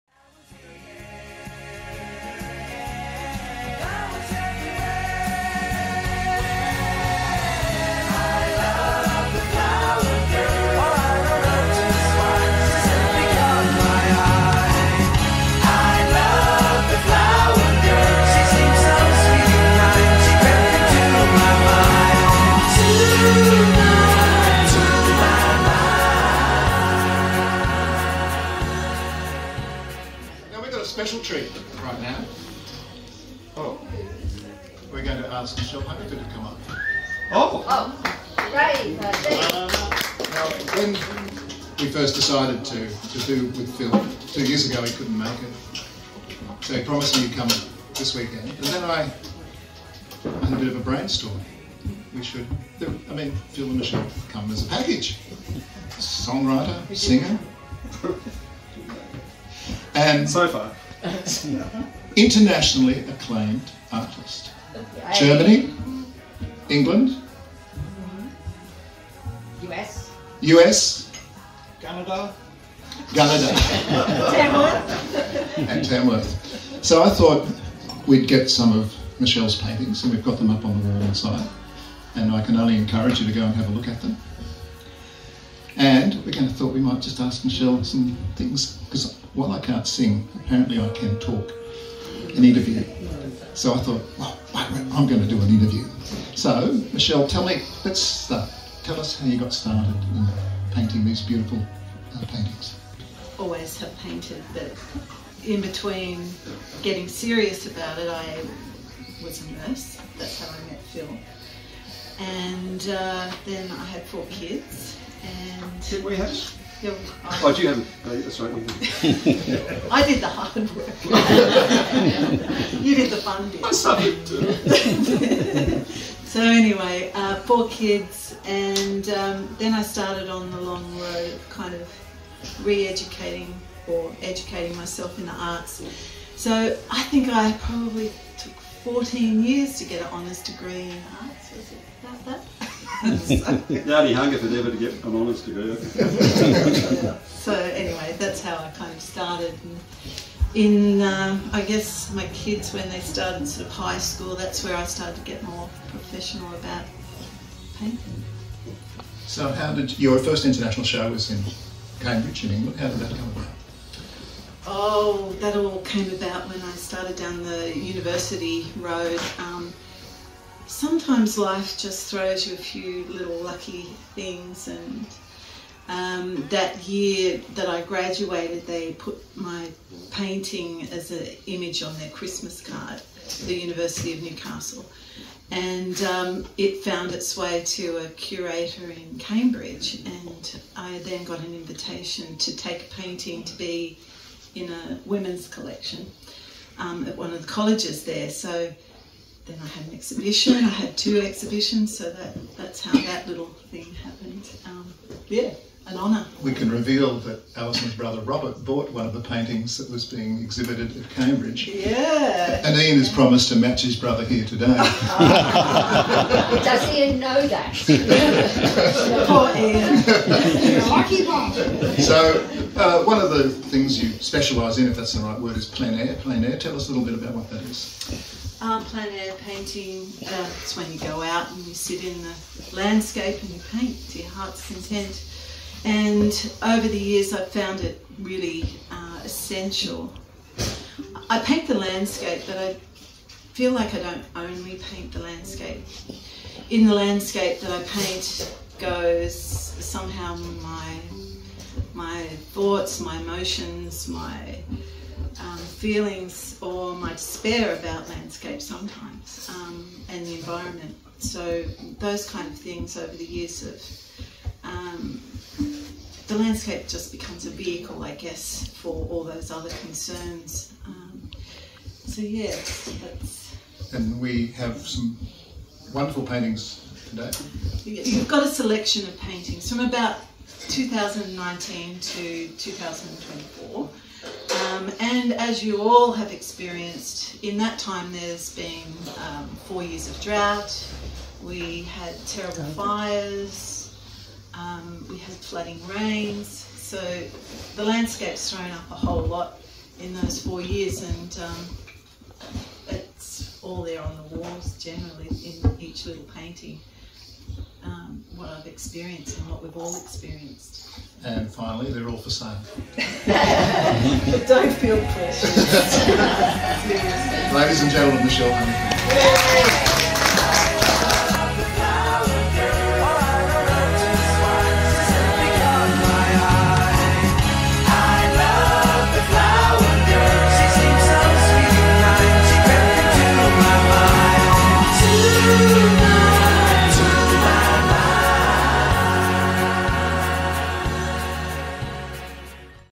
MICHELLE HUNGEFORD - the Interview
World recognised artist Michelle Hungerford talks about what inspires her paintings at Loaves and Fishes on The Deck on Good Friday, 2024